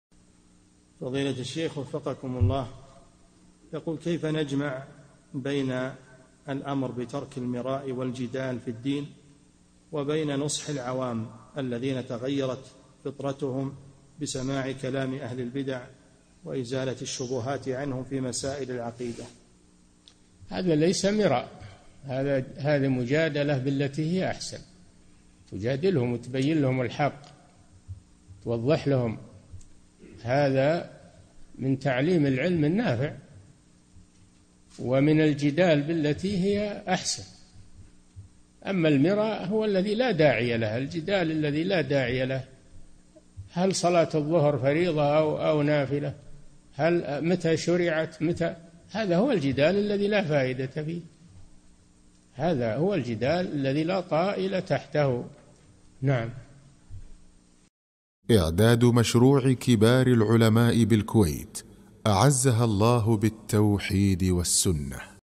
[558 -582] كيف يجمع بين النهي عن المراء والجدال وبين جدال العوام الذين تأثروا بكلام أهل البدع؟
-"السؤال: كيف نجمع بين الأمر بترك المراء والجدال في الدِّين وبين نصح العوام الذين تغيرت فطرتهم بسماع كلام أهل البدع وإزالة الشبهات عنهم في مسائل العقيدة؟
الجَوَابُ: هذا ليس مراء، هذه مجادلة بالتي هي أحسن تجادلهم تبين لهم الحق وتوضح لهم هذا من تعليم العلم النافع ومن الجدال بالتي هي أحسن أما المراء فهو الذي لا داعي له الجدال الذي لا داعي له، هل صلاة الظهر فريضة أو نافلة؟ متى شرعت؟ متى...؟ هذا هو الجدال الذي لا فائدة فيه، هذا هو الجدال الذي لا طائل تحته.
للاشتراك في قناة الشيخ صالح الفوزان
سؤال وجواب من شرح صفة الصلاة ( 582 ) مقطع

للاستفسار والإقتراحات والدعم
رئيس الفريق
لمشروع كبار العلماء